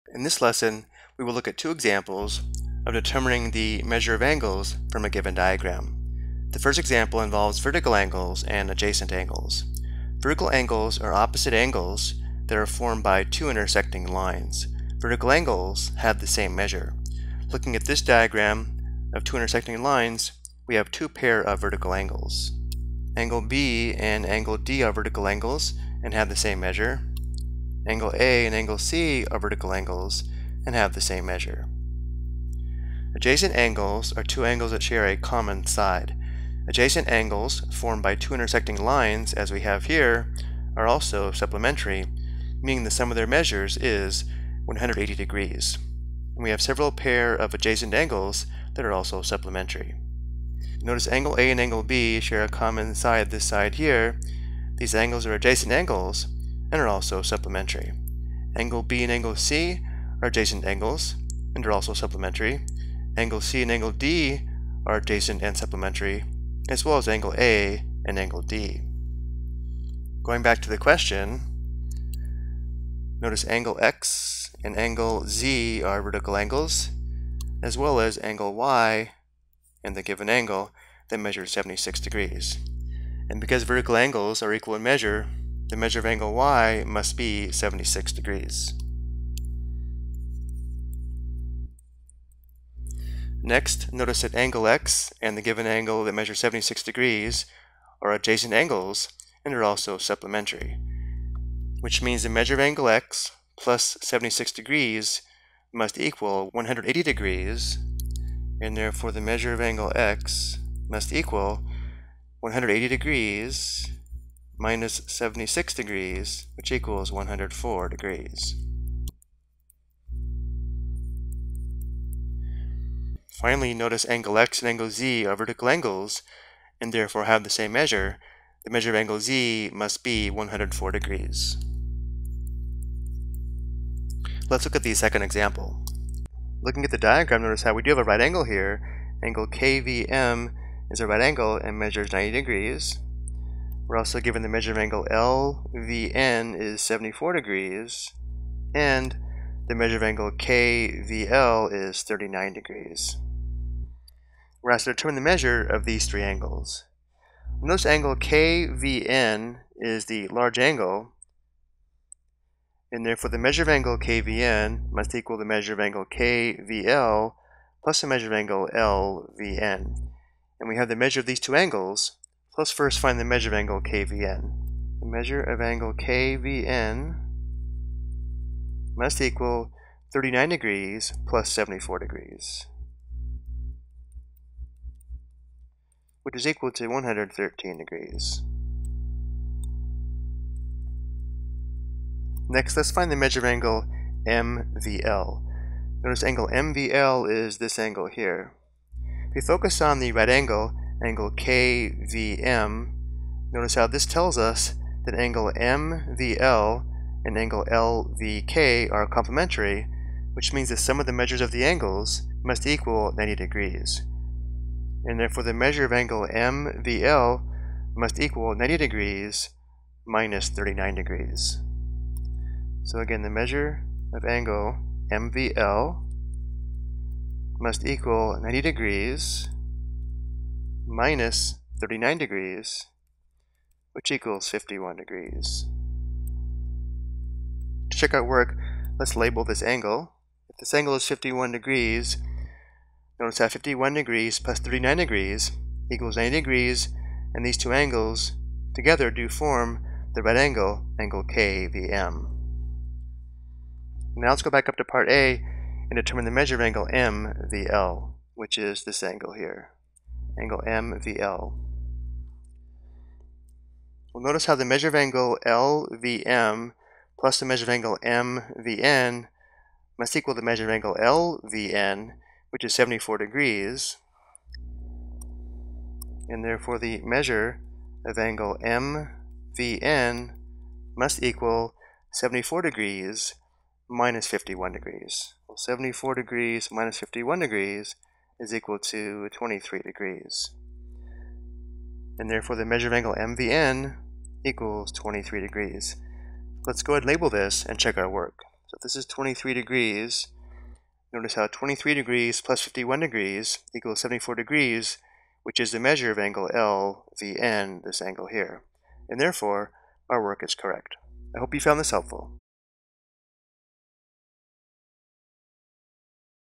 Determine the Measure of Vertical Angles and Angles from a Figure
This video explains how to determine the measure of vertical angles and missing angles from a figure.